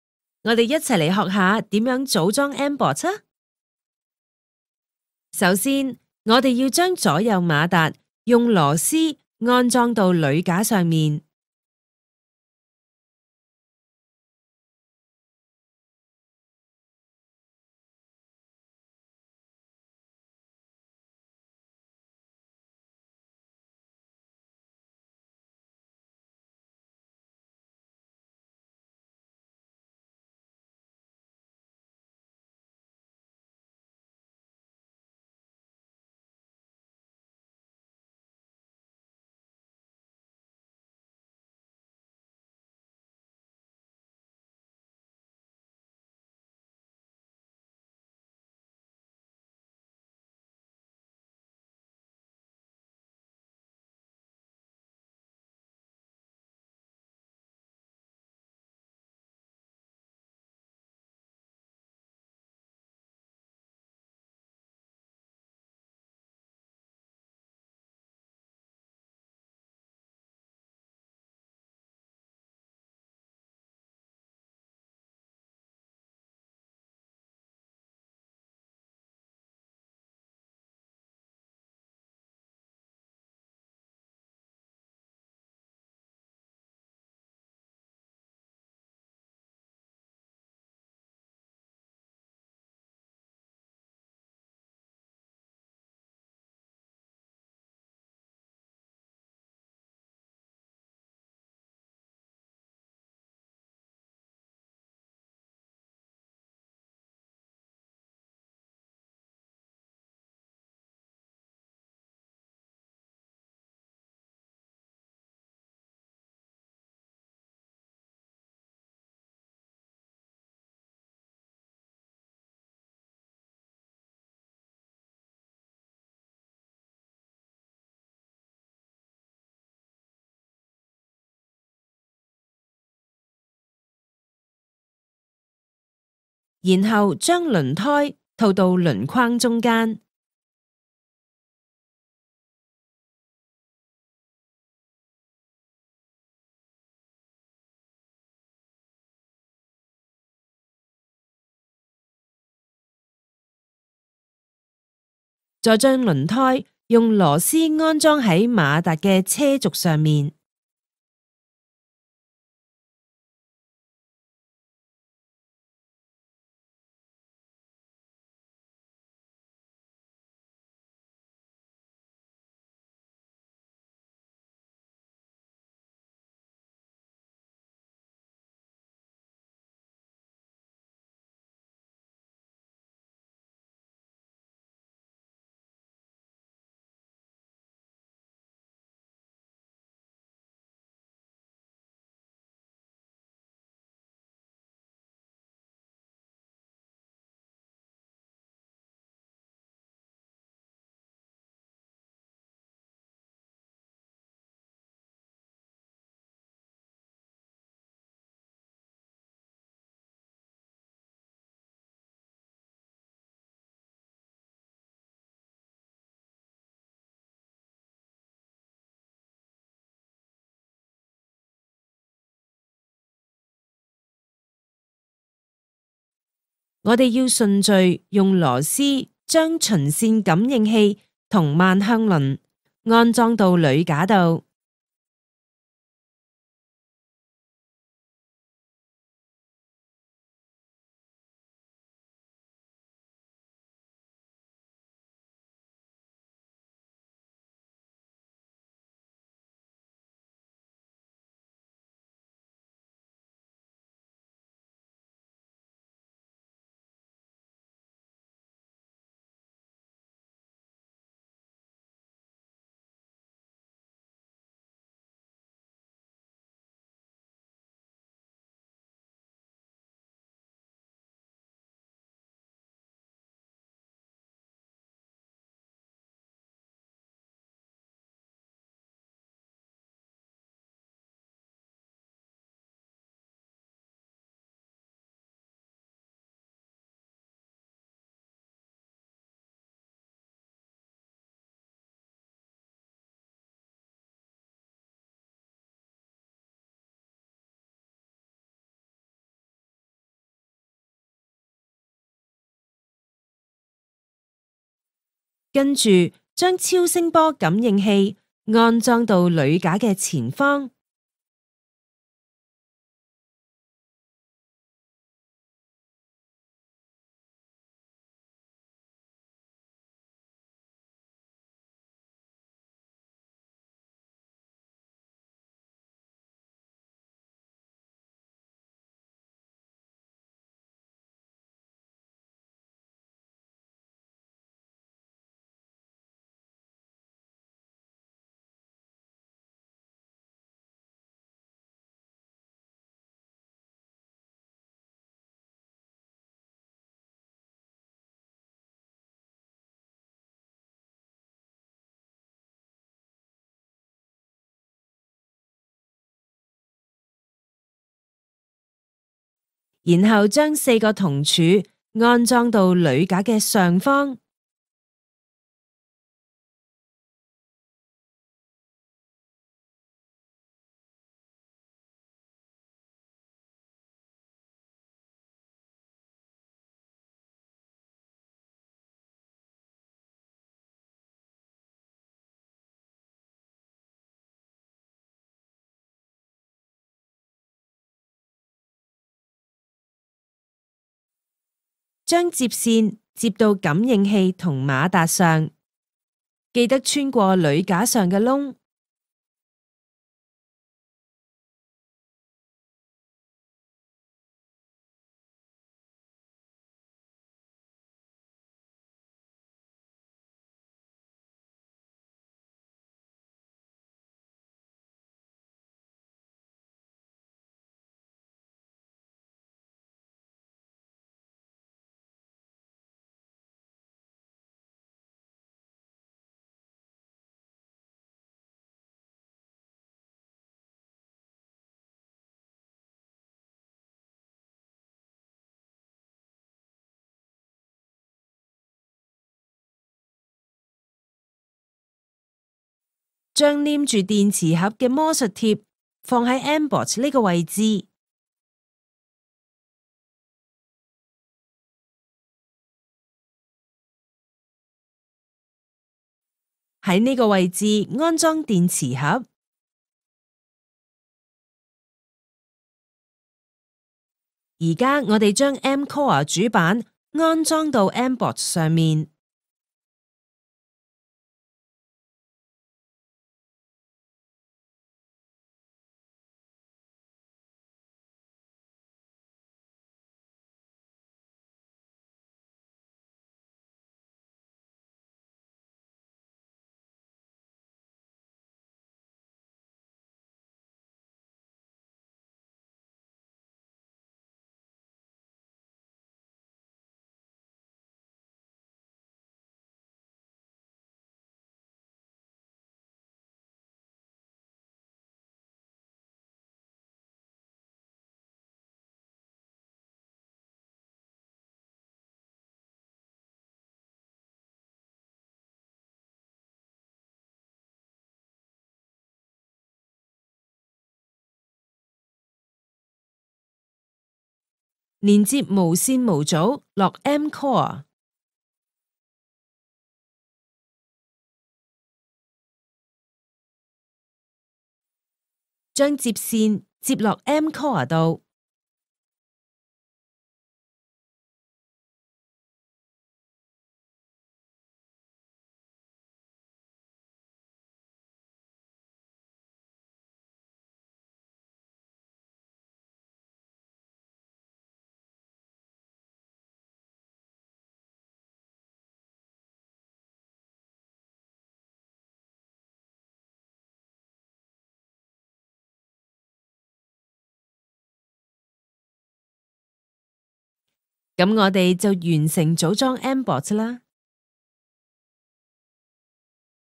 機械人製作大師：認識組裝mBot的方法
大家一起來組裝mBot 吧！